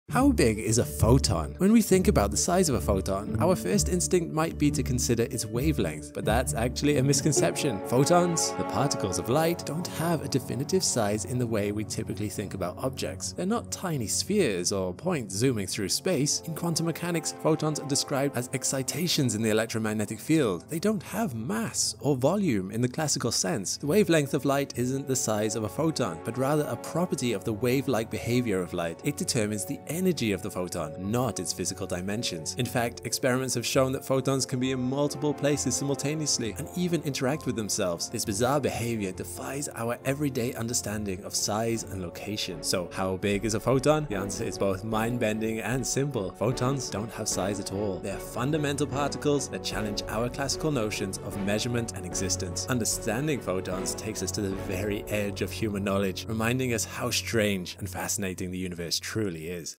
How Big Is A Photon?
Have you ever wondered how big a Photon really is? Let me tell you.